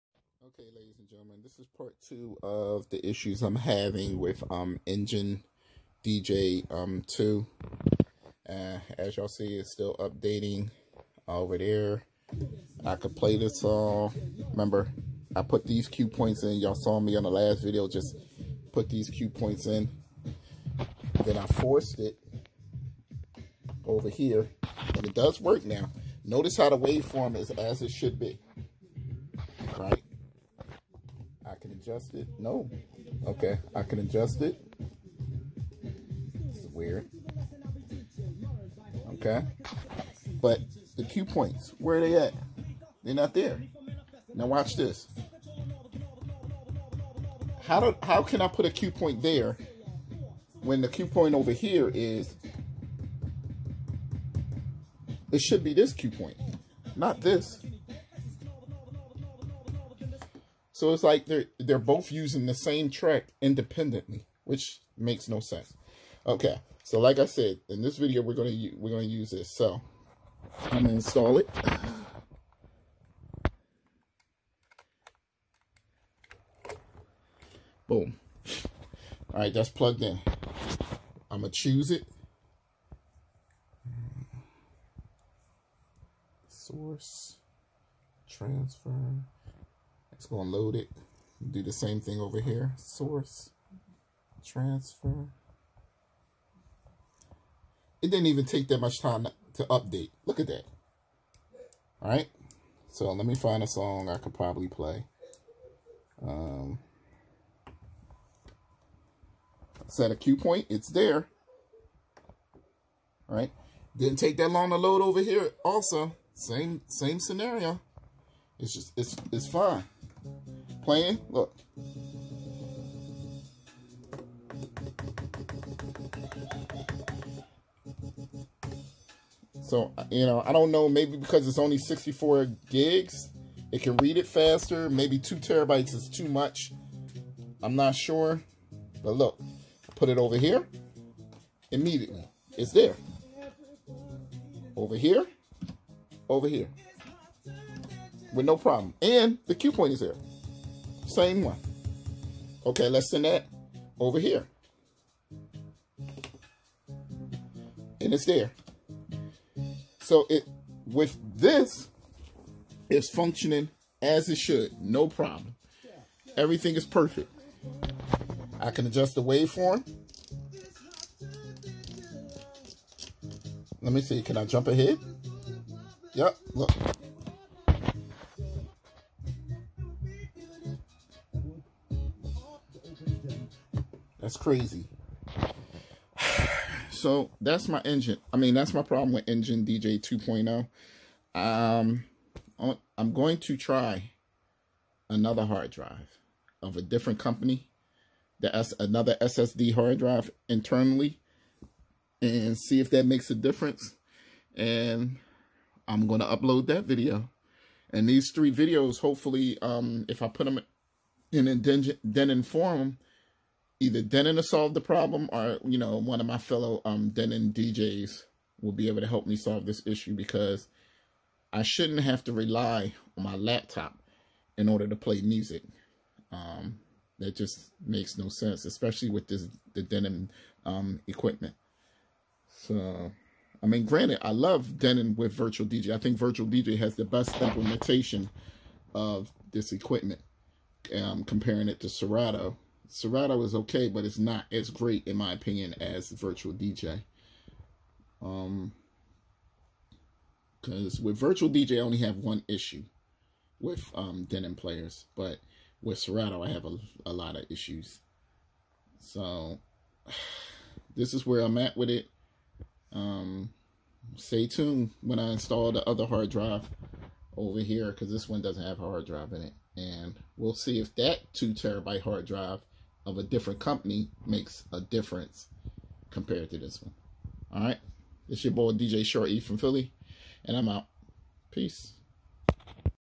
Problems with EngineDJ Denon Prime..Part two.
using enginedj 2.0.2